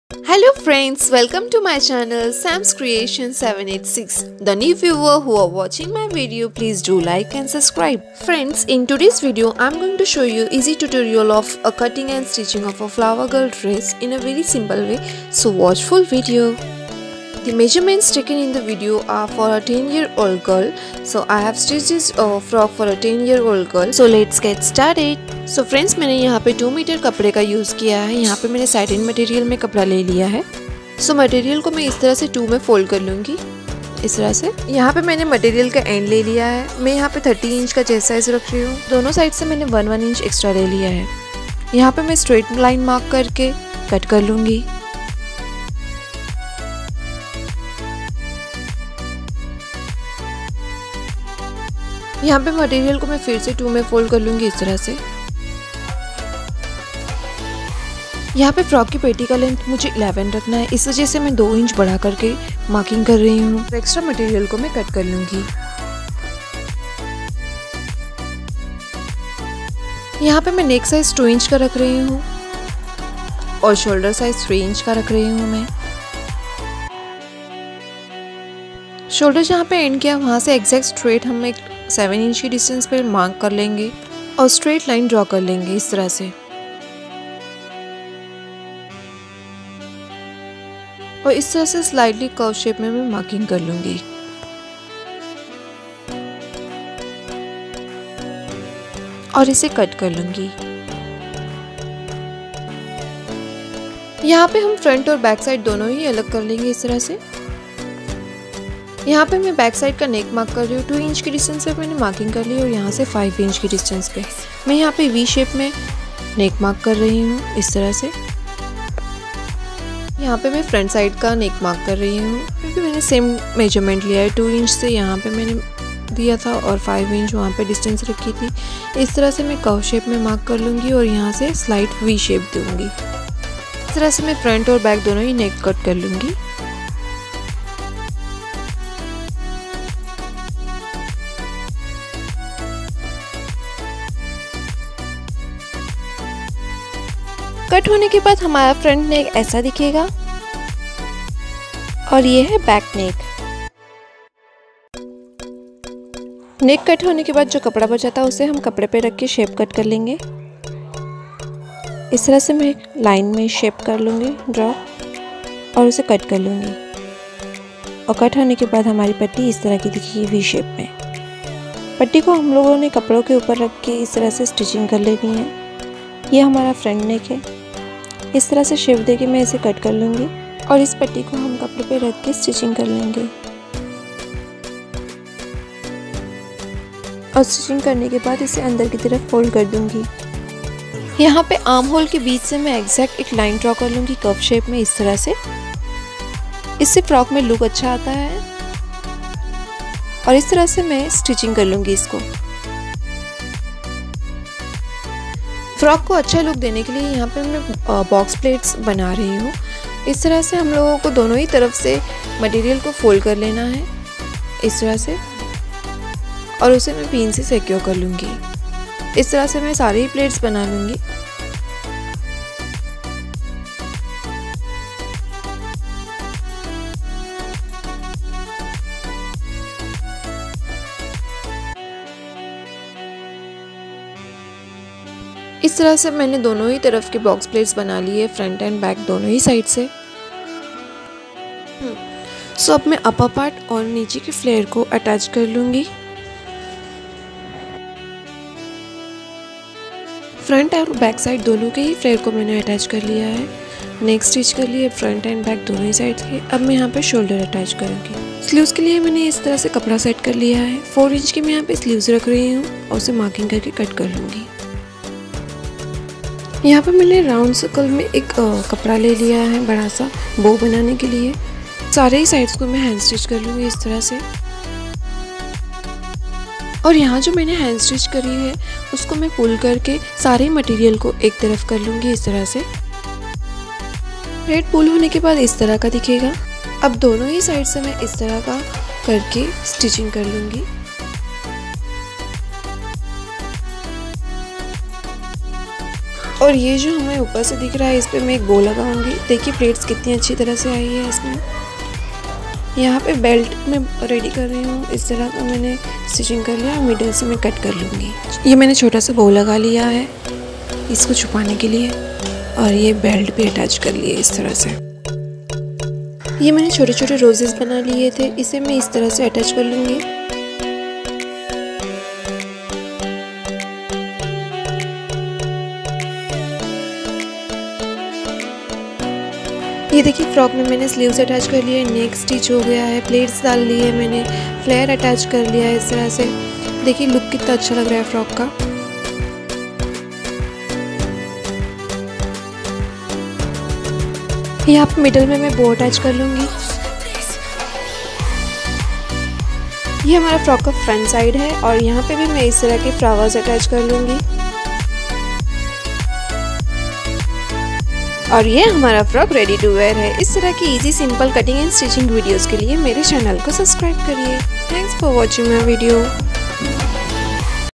DIY Designer Box Pleated flower girl Frock cutting & stitching full tutorial
@sam's creation 786

Box pleated frock
designer frock
baby frock
latest frock
flower girl frock
satin frock
circular frock
simple  easy frock cutting

In today's video I m going to show easy tutorial of box pleated frock in very simple and easy way so u can try it at home

If u have any queries regarding video pls ask me in comment.

Thanks for ur special visit😊